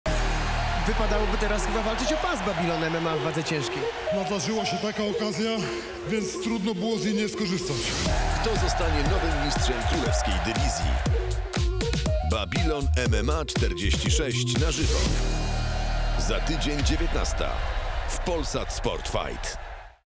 🏆 TRAILER Babilon MMA 46: Marcin SIANOS vs Oli THOMPSON
🔥 Już 9 sierpnia gala Babilon MMA 46 w Międzyzdrojach z walką wieczoru o mistrzowski pas w królewskiej kategorii 🏆 Marcin SIANOS vs Oli THOMPSON - kto zwycięży i zdobędzie tytuł❓